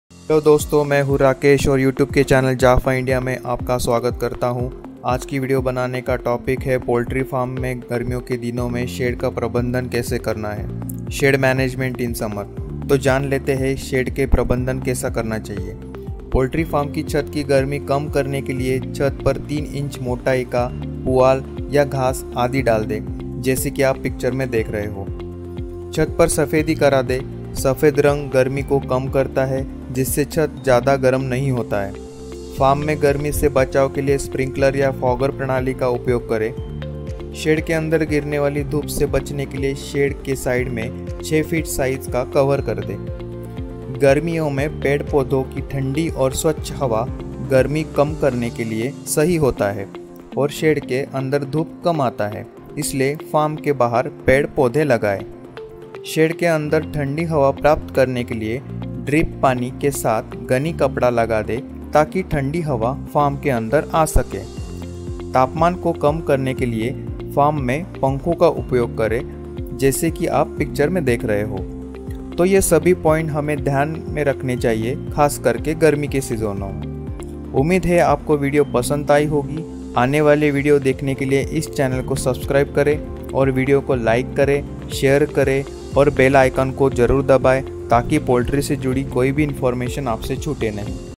गर्मी के दिनों में पोल्ट्री फार्म शेड को ठंडा कैसे रखें I
Shed के छत्त पर सुखी घास डालदे, shed को सफेदी करा दें क्योंकि सफ़ेद रंग गर्मी कम करता है. ऐसे और गर्मी से bird को बचने के लिए नुस्खे जानने हो, तो पूरा video ध्यान से देखें और आपकी राय comments में जरूर से लिखें |